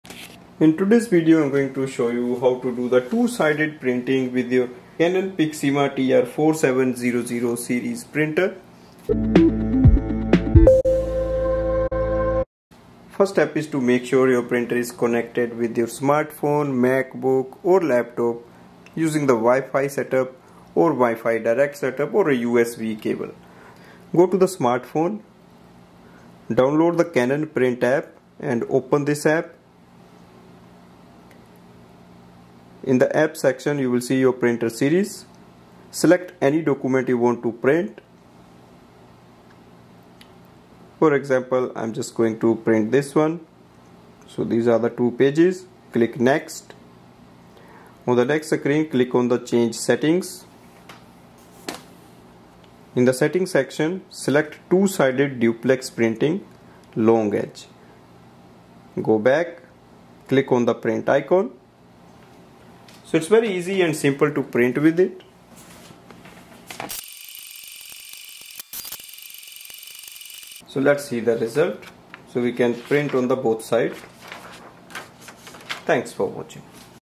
Canon Pixma TR4700, 4720, 4722, 4723, 4750i, 4751i 2-Sided Copy.
This video reviews how to do the 2-sided copy with the Canon Pixma TR4720, TR4722, TR4723, TR4750i and TR4751i All-in-one inkjet printer.